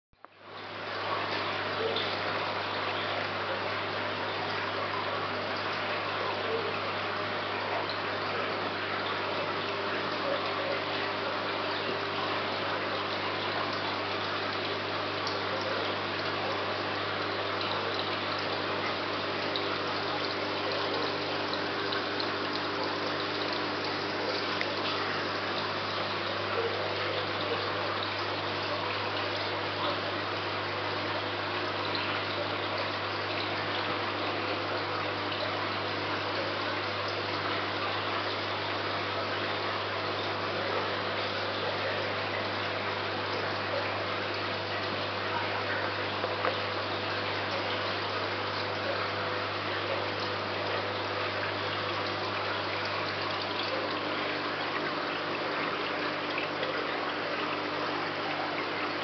Tanganyika 120l. base 80x50cm.
Pareja de Neolamprlogus buescheri y trio 1/2 Neolamprologus meleagris.